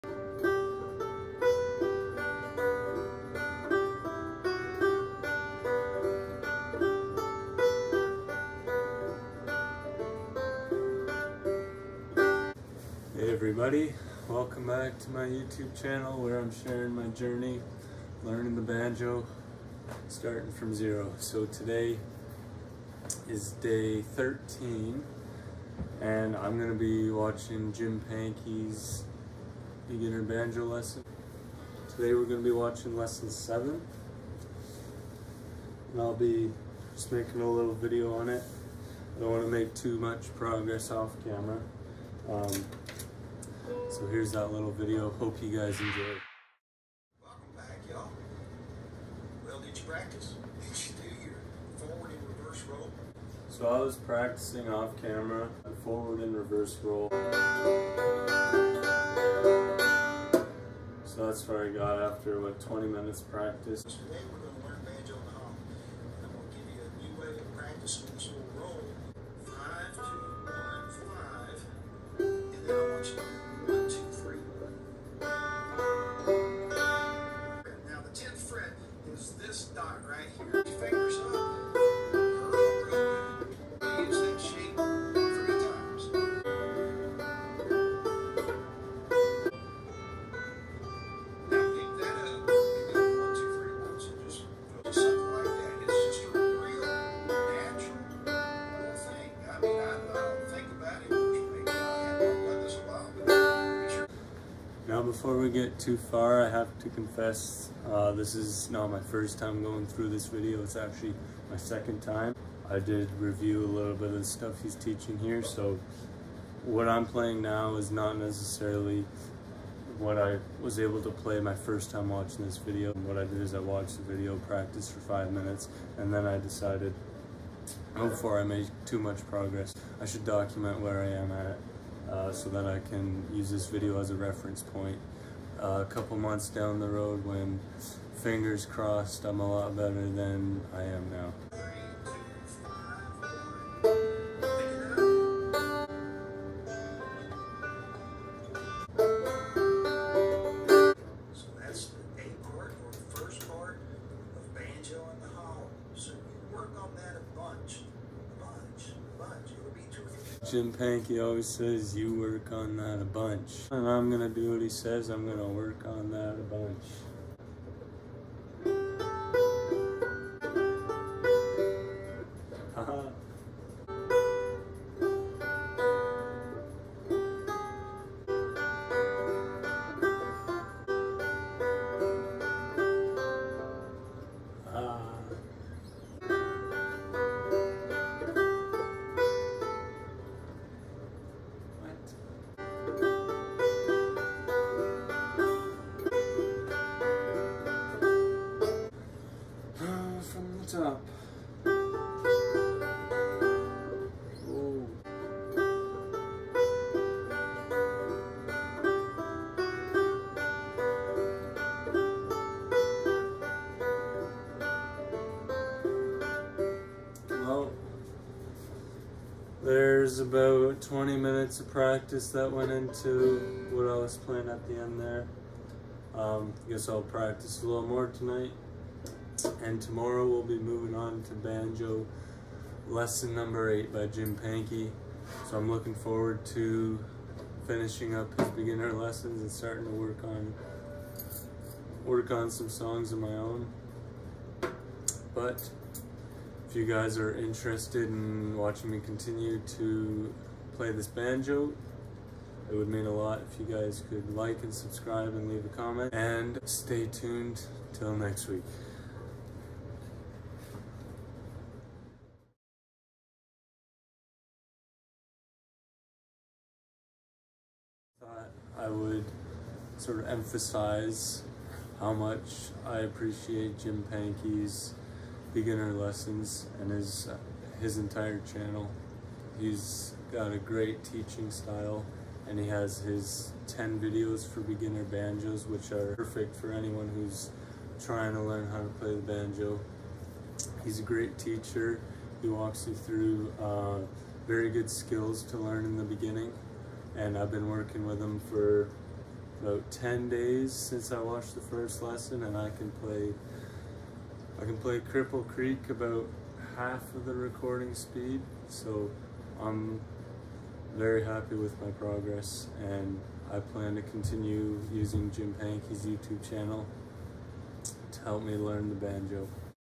LEARNING THE BANJO!!! (Lesson 7)
In this video, I share my practice session for lesson 7 of learning the banjo!

I highly recommend Jim Pankey's Channel for anyone else who is learning to play the banjo:

If you have ever wanted to learn how to play the banjo, then you and I have something in common! Recently I finally made the decision to purchase my very first banjo and am looking forward to learning to play through YouTube tutorials.